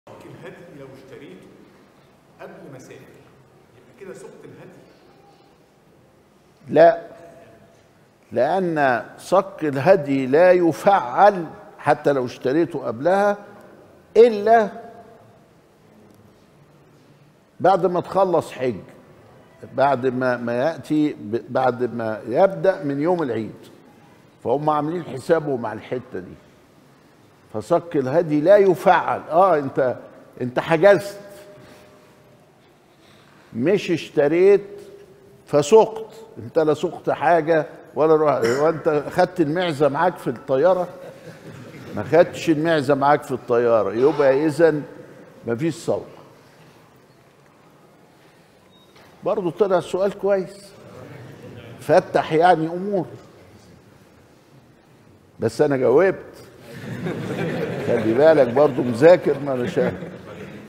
سؤال من أحد الحاضرين : هل أعتبر أنني سقت الهدي بشراء صك الهدي ؟ | شرح متن الغاية والتقريب
المجلس الثامن والأربعون من شرح متن الغاية والتقريب | أ.د علي جمعة